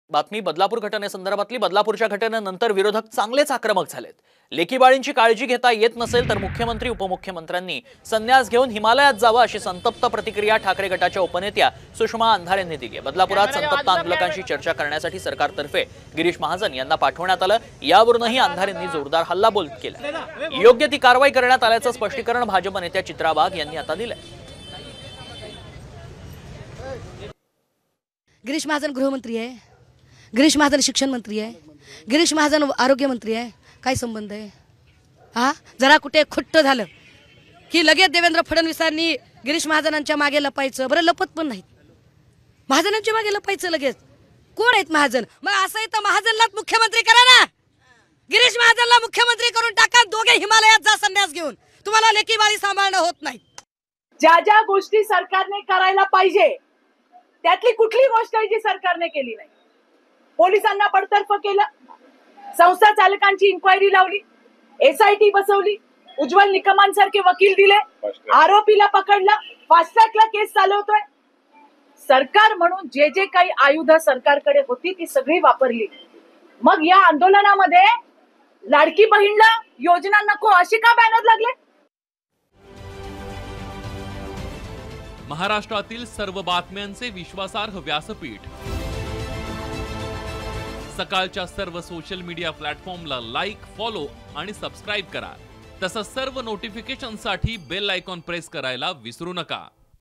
Chitra Wagh सरकारने ज्या ज्या गोष्टी करायच्या त्या सगळ्या केल्यात । Sushma Andhare ।badalapur news ।
शिवसेना ठाकरे गटाच्या नेत्या सुषमा अंधारे आज बदलापुरात परिस्थितीची पाहणी करण्यासाठी गेल्या होत्या. त्यावेळी त्यांचा मोर्चा पोलिसांनी अडवला, त्यानंतर अंधारेंनी रस्त्यातच ठिय्या मांडला. यावेळी त्यांनी मुख्यमंत्री शिंदे आणि उपमुख्यमंत्री फडणवीसांवर टीका केली. त्यावर आता चित्रा वाघ यांनी उत्तर दिले आहे.